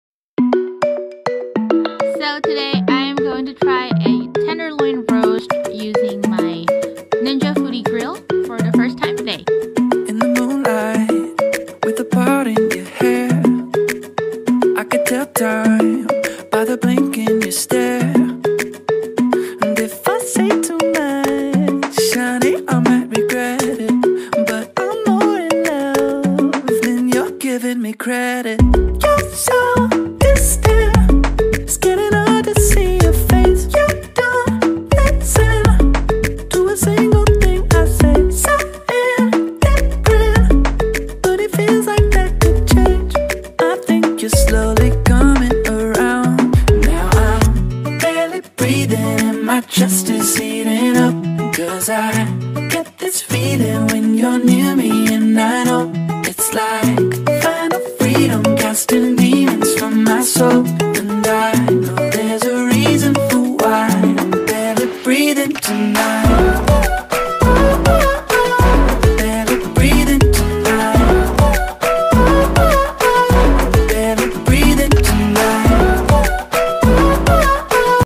Life with Kimi ♥ My First Ninja Foodie Grill Tenderloan
Hey loves! Welp, this is my first time trying to make pork tenderloin using the bake function on my Ninja Foodie Grill XL. Not bad!